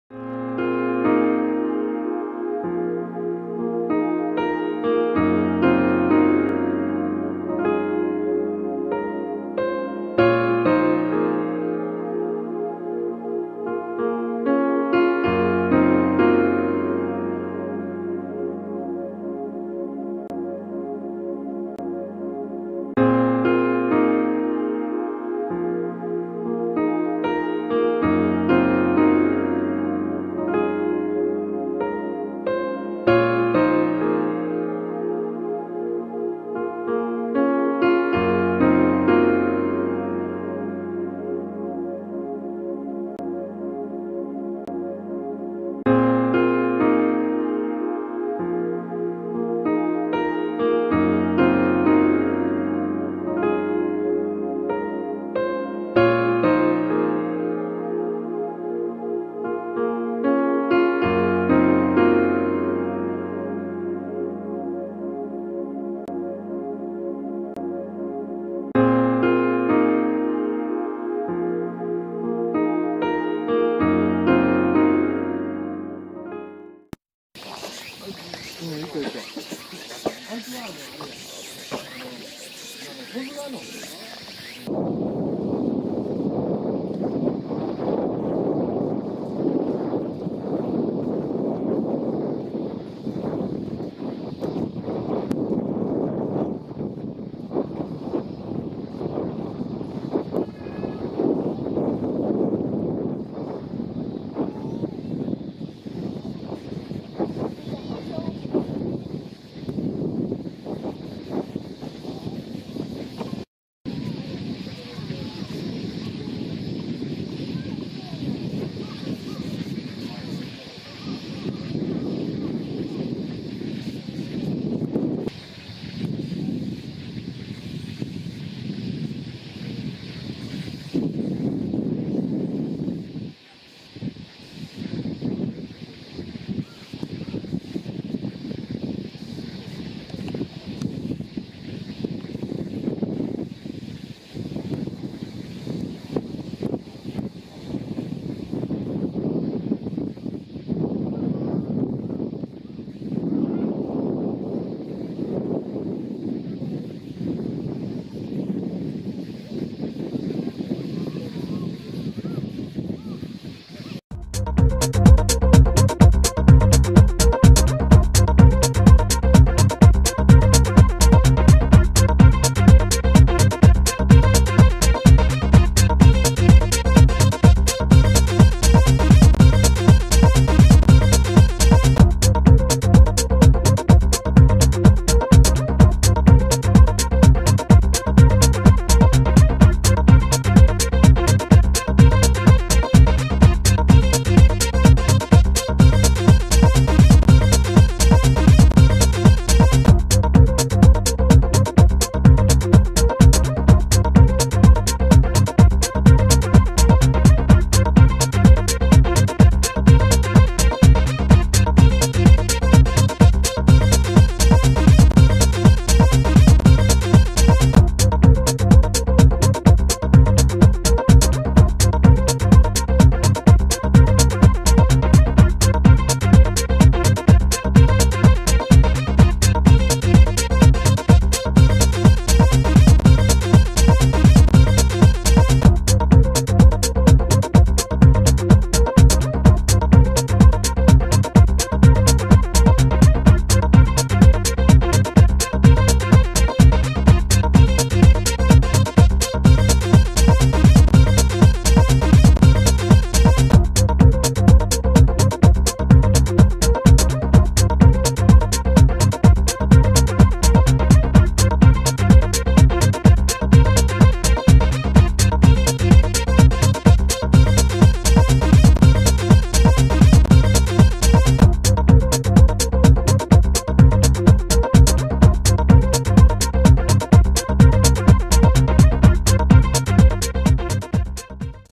星のブランコ（ほしだ園地） Star Blanco in Japan
奈良と大阪の境にあるほしだ園地に行ってきました。星のブランコと命名された吊り橋を中心にまとめてみました。最後にクライミングウォールの様子を撮影。次回作品は、クライミングウォールを中心に現在、製作中！